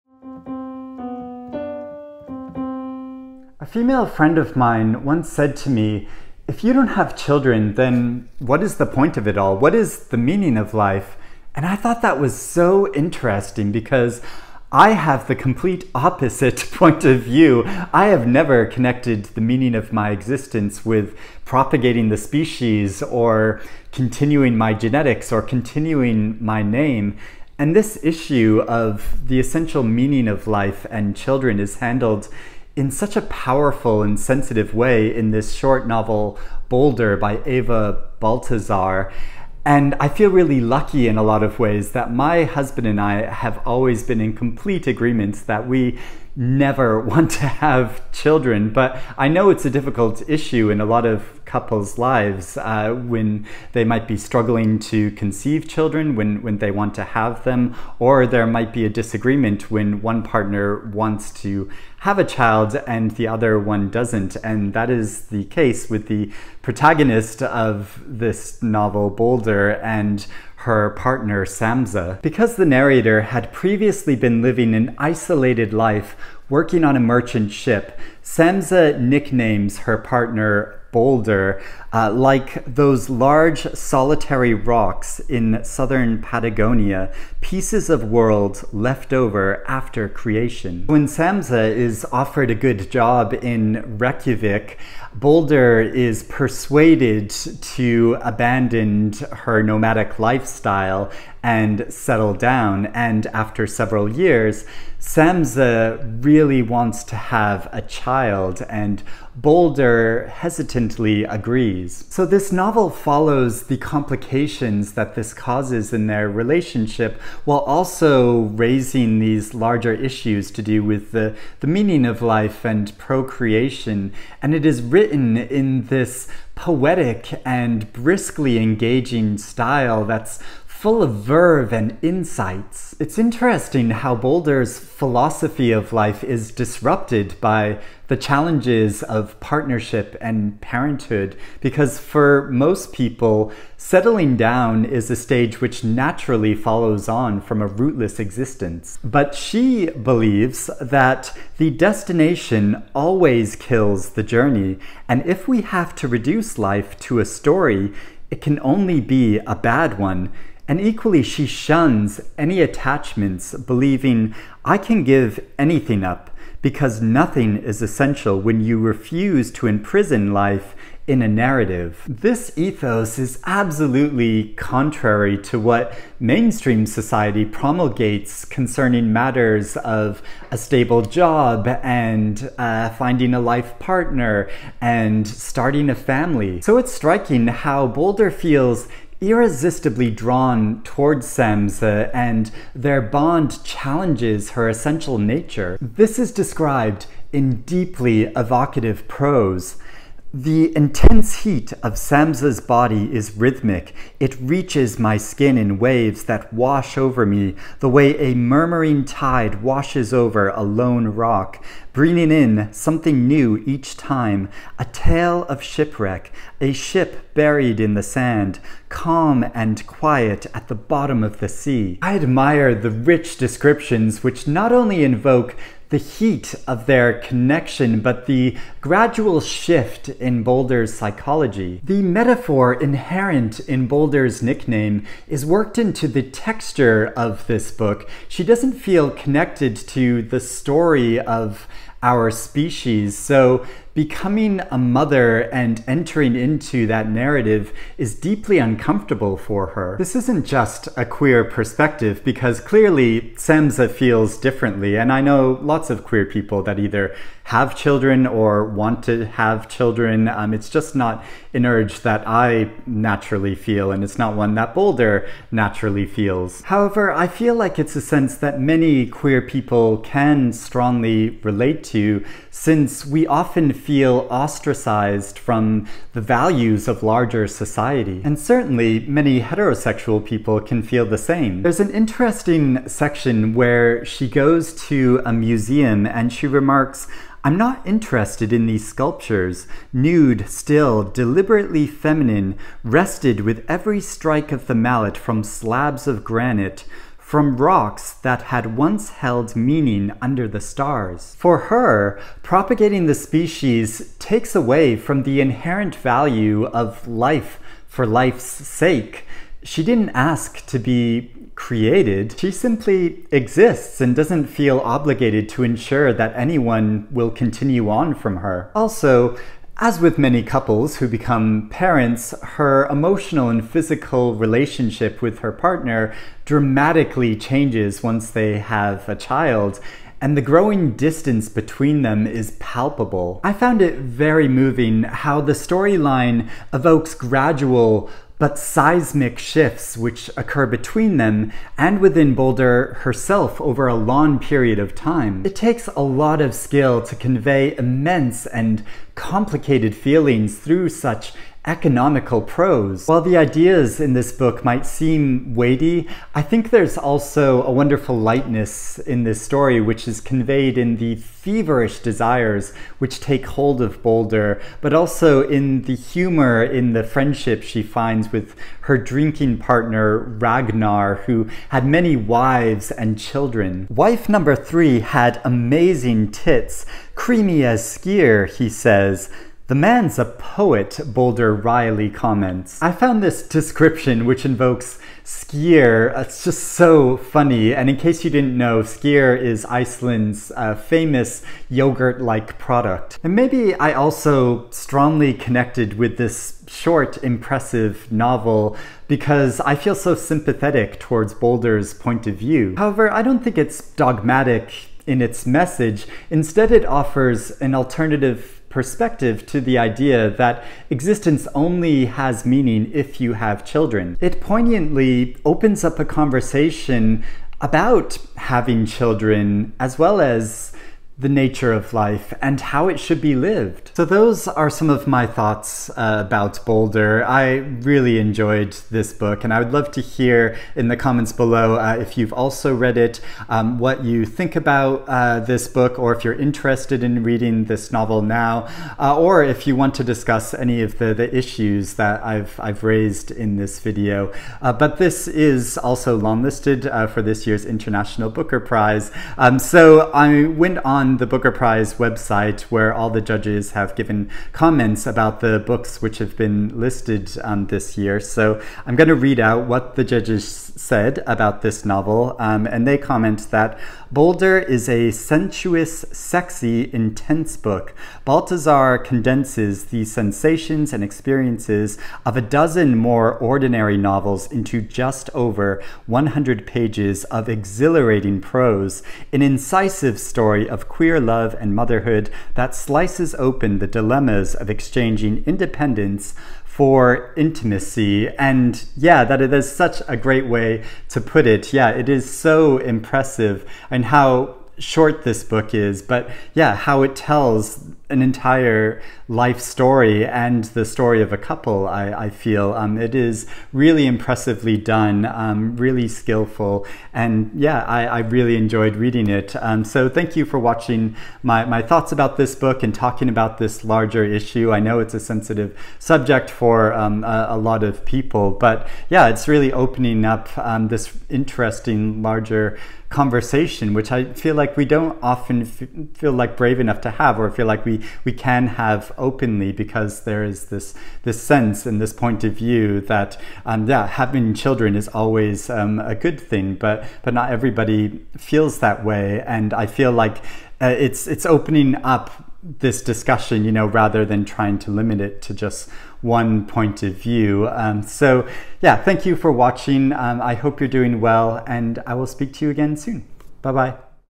Boulder by Eva Baltasar / Review
Is it necessary to have children in order to find meaning in life? That's a question which is sensitively explored with a lot of insight in this short novel. Click ‘Show More’ for info & links.